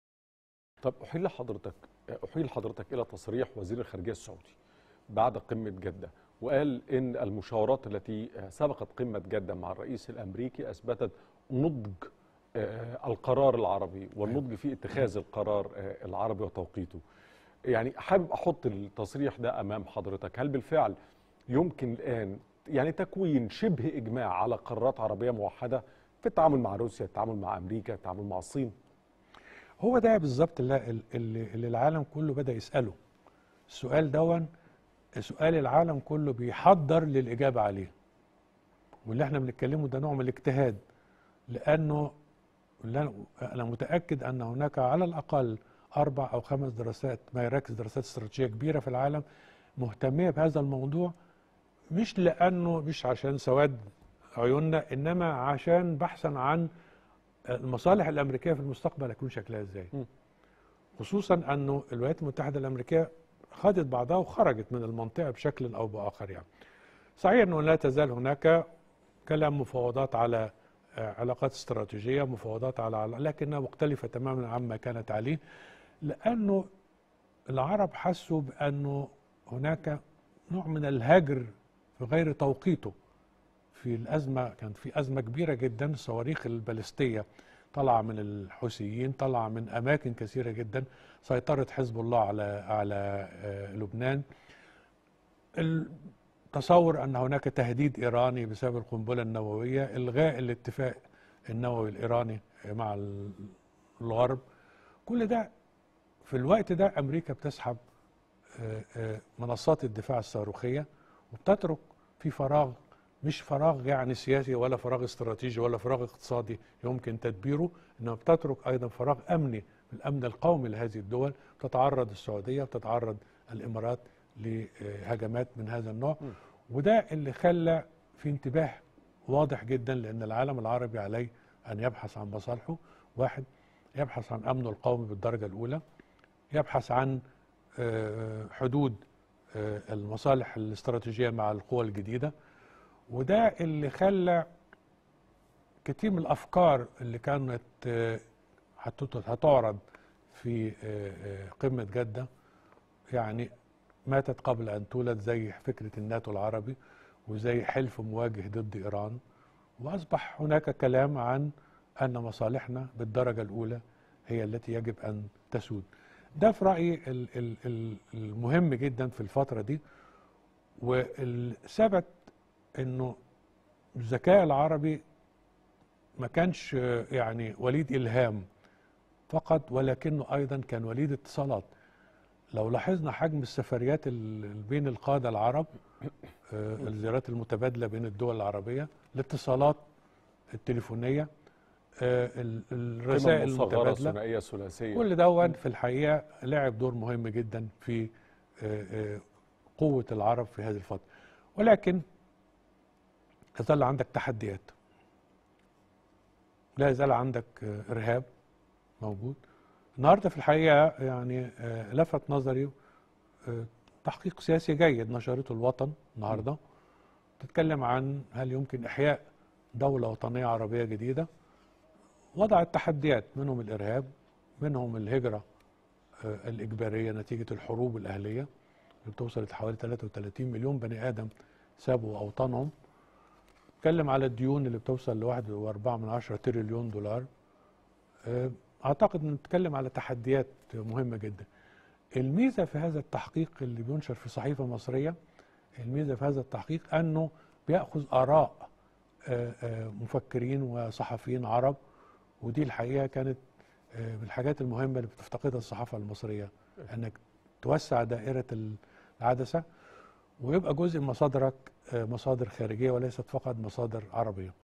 بعد قمة جدة.. الكاتب الصحفي عادل حمودة يتحدث عن نضج القرار العربي واتحاده في مواجهة الأزمات العالمية
من مصر | بعد قمة جدة.. الكاتب الصحفي عادل حمودة يتحدث عن نضج القرار العربي واتحاده في مواجهة الأزمات العالمية